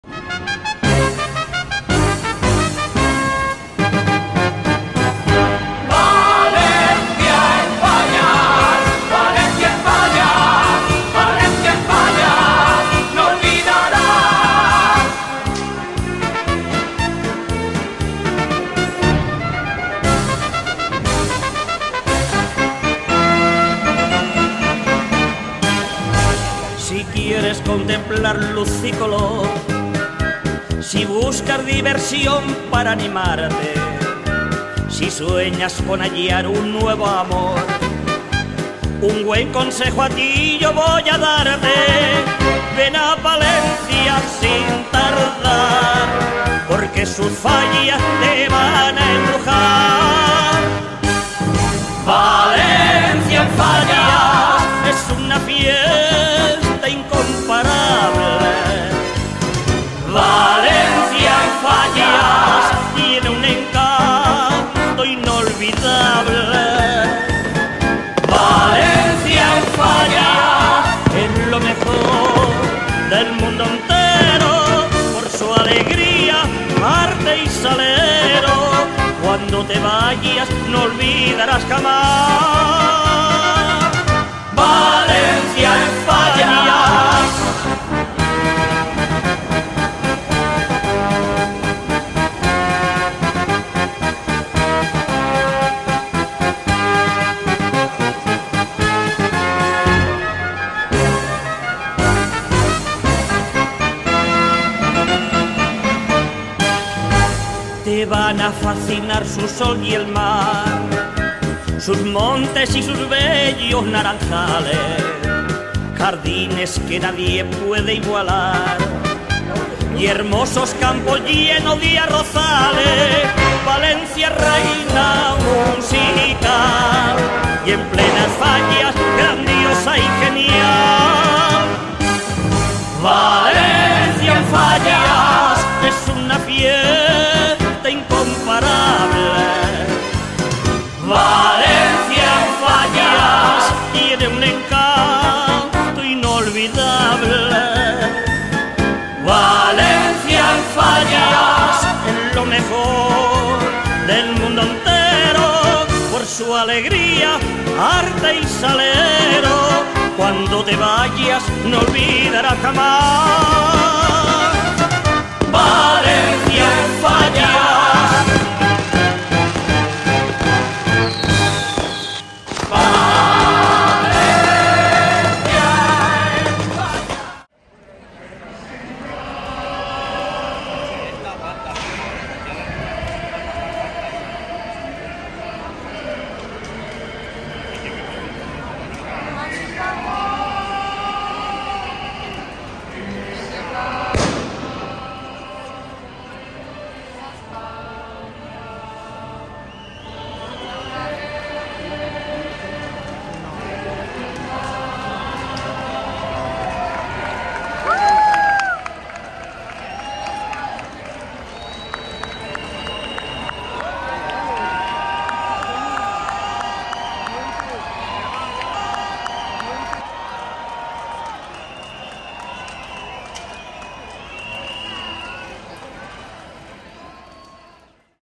Valencia en Fallas 2022
A video capturing Valencia during famous Fallas festival. Scenes of burning sculptures representing year passed can be seen throughout the city. During the festival there are daily firework shows called La Mascleta.

Captured exclusively using iPhone 13 Pro at 4k 60fps.